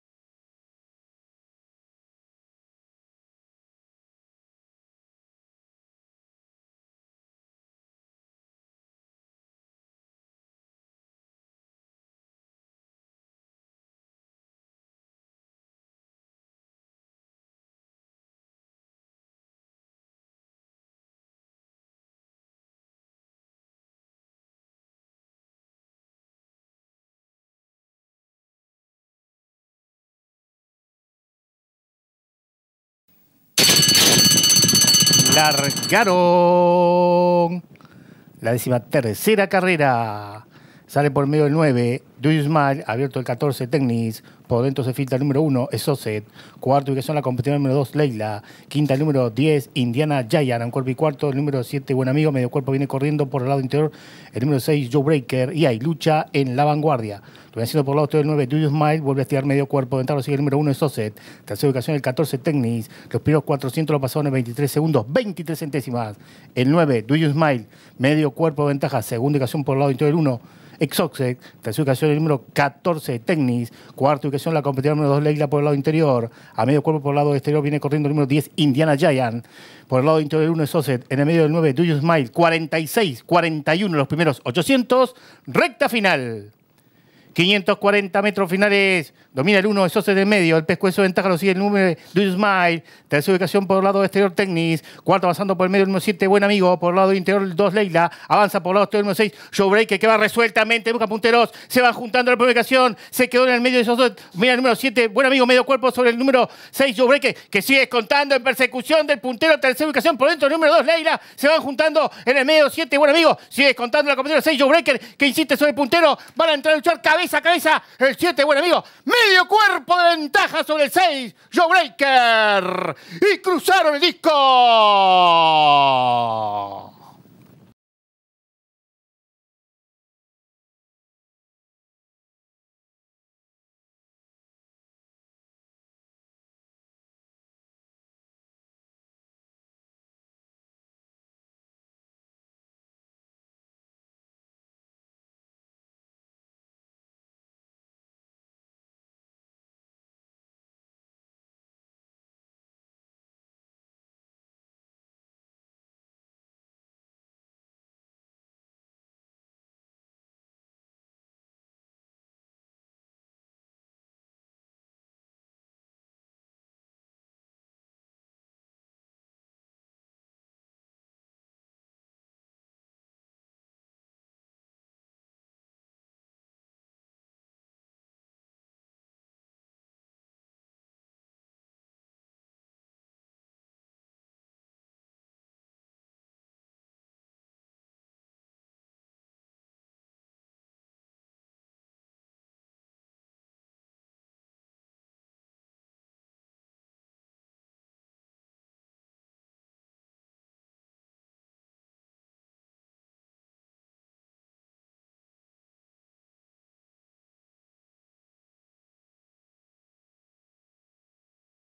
13/08/2017 – Hipodromo Maroñas – Carr 13 – LADY LILITH (2015)
Carrera 13 realizada el dia 13/08/2017 por el premio LADY LILITH (2015) en el Hipodromo Maroñas